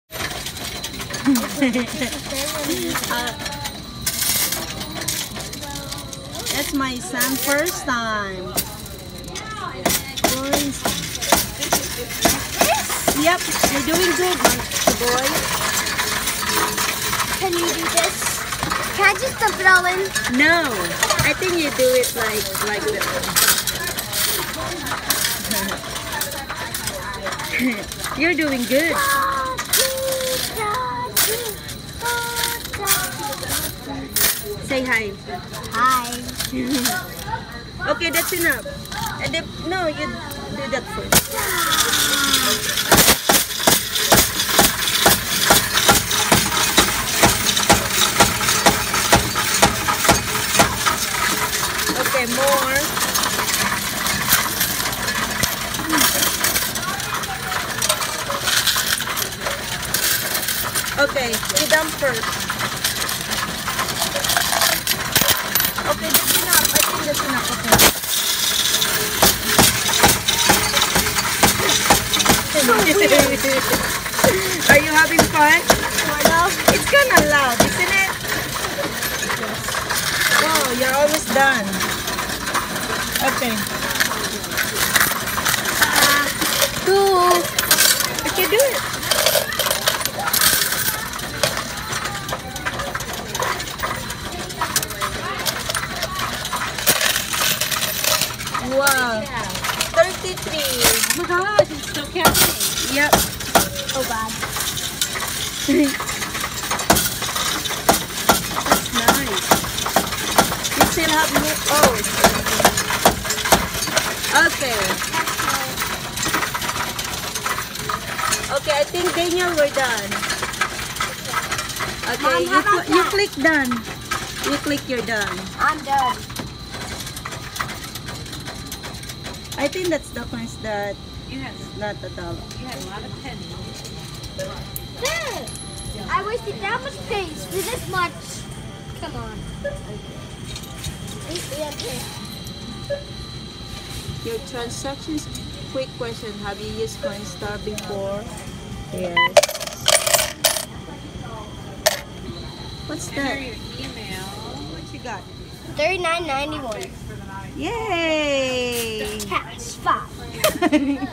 Our Little Boy Daniel Visits The Coinstar Machine How Much Will He Have
Daniel decided to cash in his change he'd been saving. How much did he have? Take a couple of minutes to watch and see.